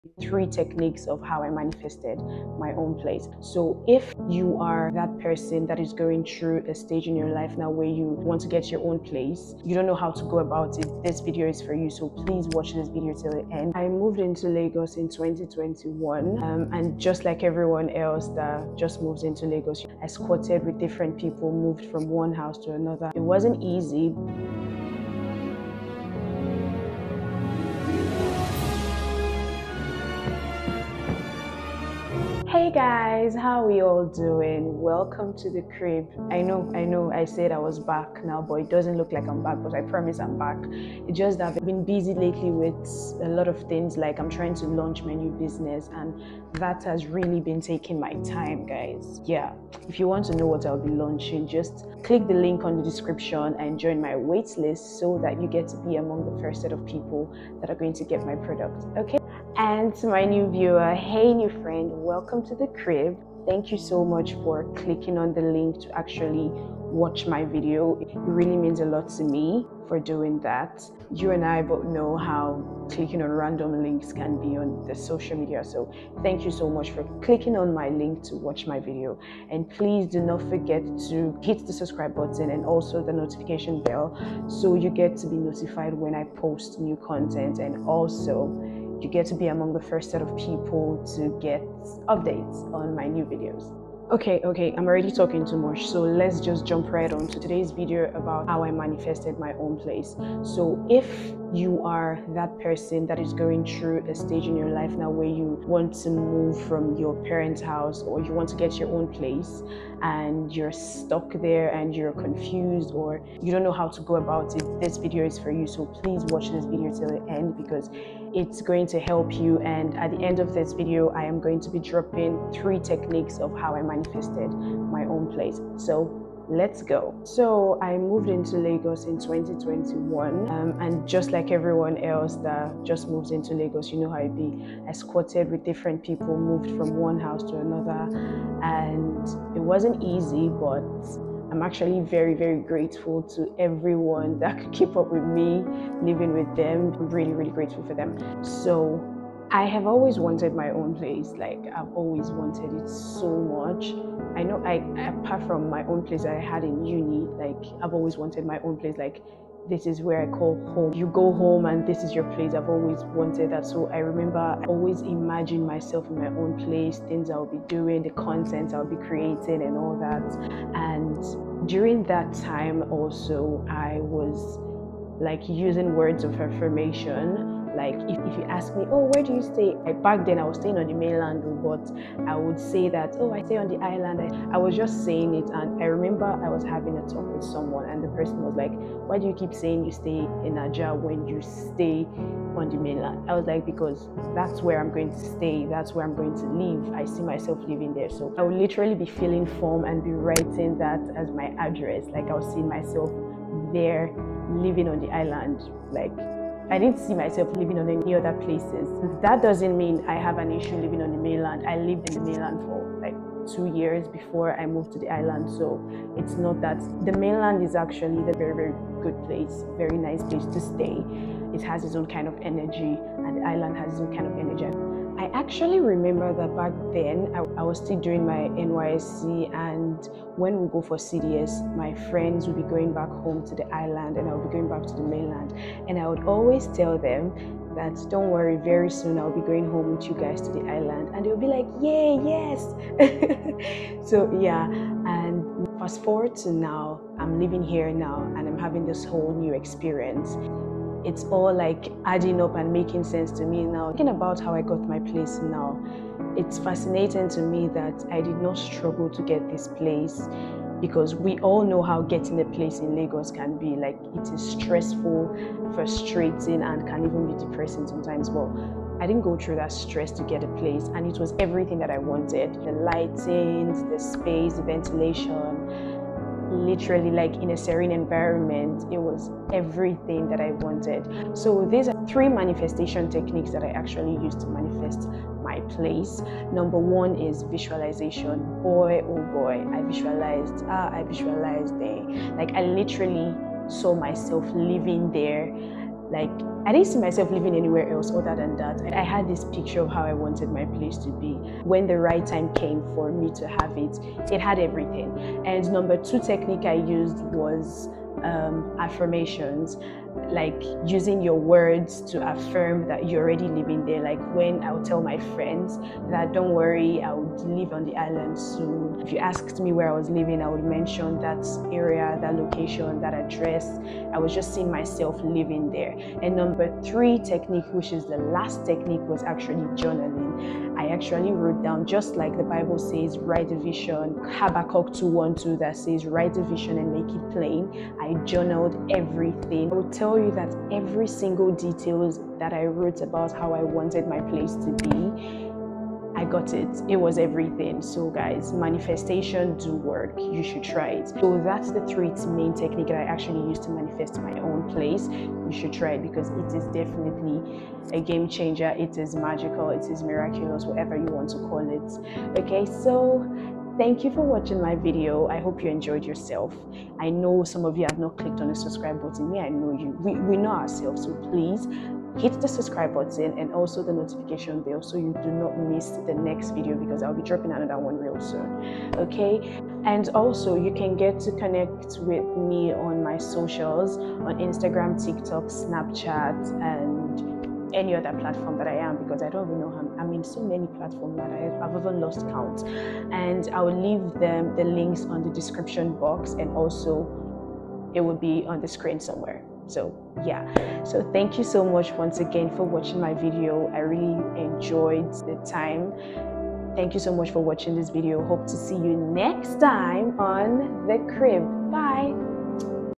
How I manifested my own place
In this video you’re going to figure out how I manifested and got my own place
00:00 Teaser

00:24 Intro

02:09 When I relocated to Lagos

04:15 Thinking about how I got my place

06: 53 Outro